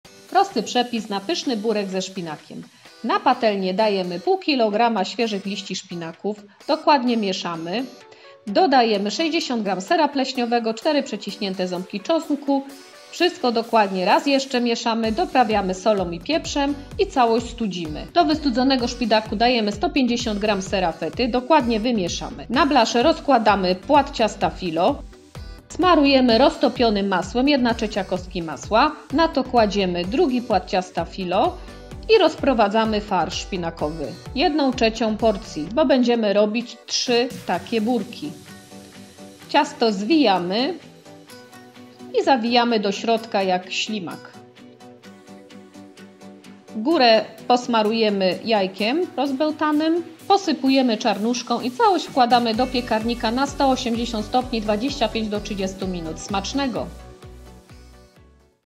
Burek ze szpinakiem. Gotowanie Po Mojemu
Burek ze szpinakiem, to danie, które chodziło już za mną od jakiegoś czasu. Kiedy wreszcie udało mi się kupić ciasto filo, to w końcu mogłam go przyrządzić. Burek ze szpinakiem i serem feta z dodatkiem sosu czosnkowego... rewelacja. Taki obiad robi się dosłownie chwilę, potem na niecałe pół godziny do piekarnika i macie coś innego niż standard. Zróbcie koniecznie, a przekonacie się o czym mówię :-)

Znajdziesz mnie także tutaj:

Pozdrawiam serdecznie,
Bożena
Gotowanie Po Mojemu